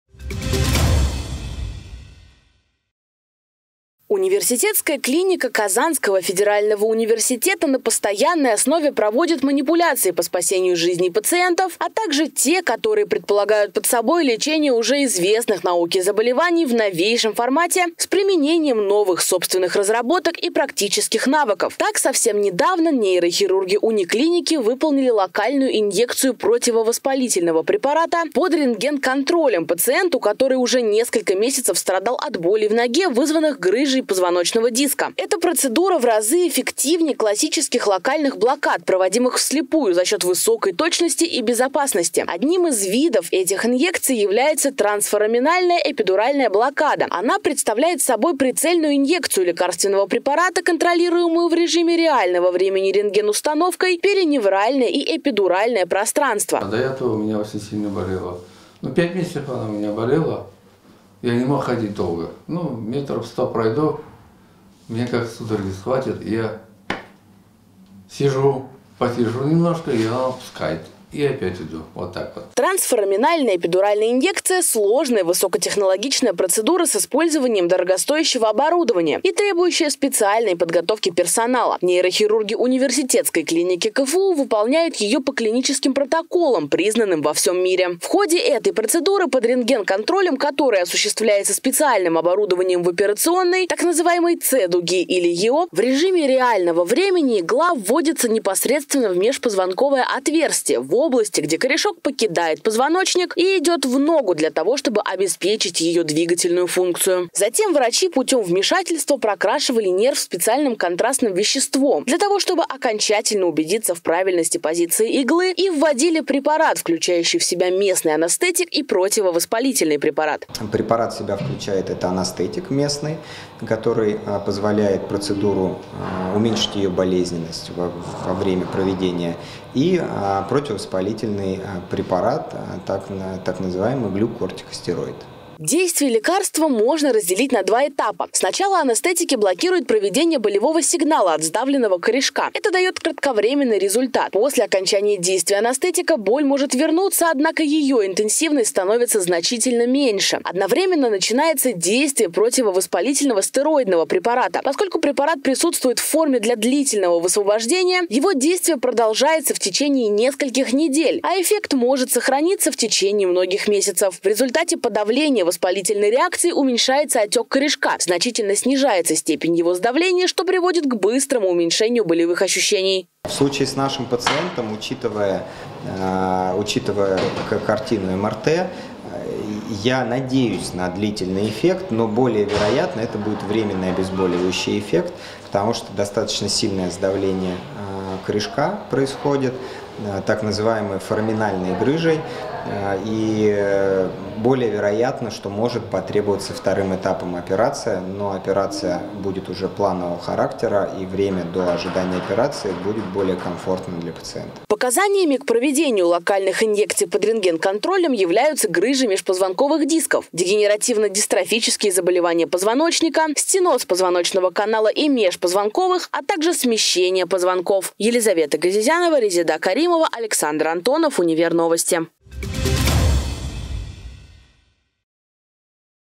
КАК ОБЛЕГЧИТЬ БОЛЬ ПРИ МЕЖПОЗВОНОЧНОЙ ГРЫЖЕ?
В Университетской клинике Казанского федерального университета провели уникальную процедуру на позвоночнике, которая даёт возможность пациентам облегчить боль при межпозвоночной грыже.